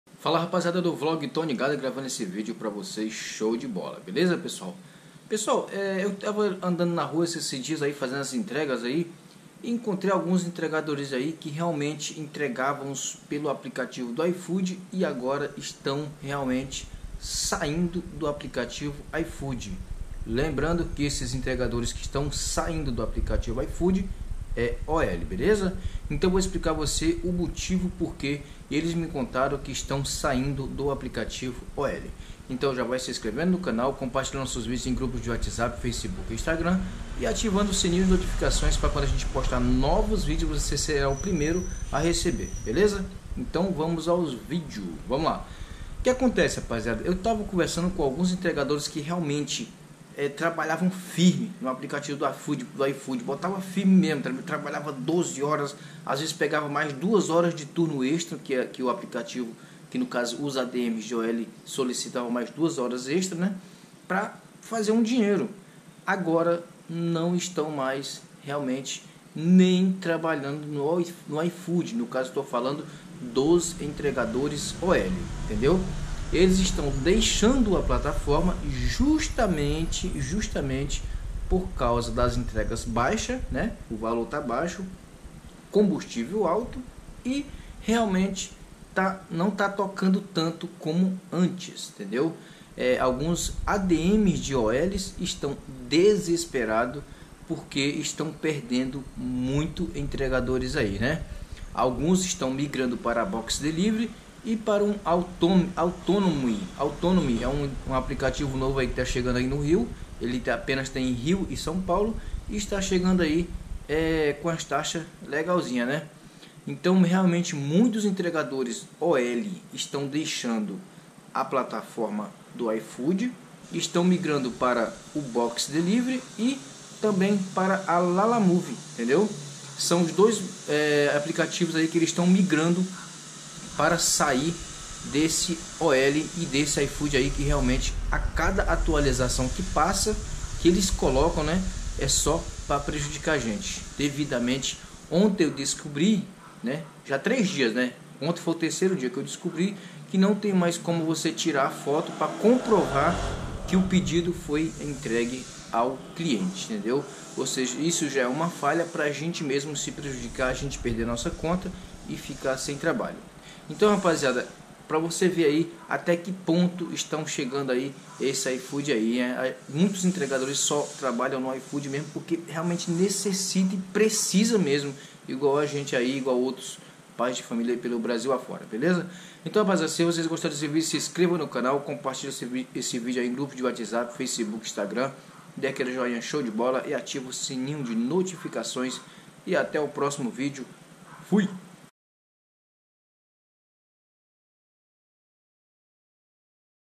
ADM OL IFOOD ESTÃO FORÇANDO ENTREGADORES FICAREM NAS RUAS VEJA NESSE VIDEO..
ADM DE OL DO IFOOD ESSTÃO PERDENDO O CONTROLE DE  ENTREGADORES, E ESTÃO FORÇANDO SEUS ENTREGADORES A FICAR A RUA VEJA NESSE VIDEO.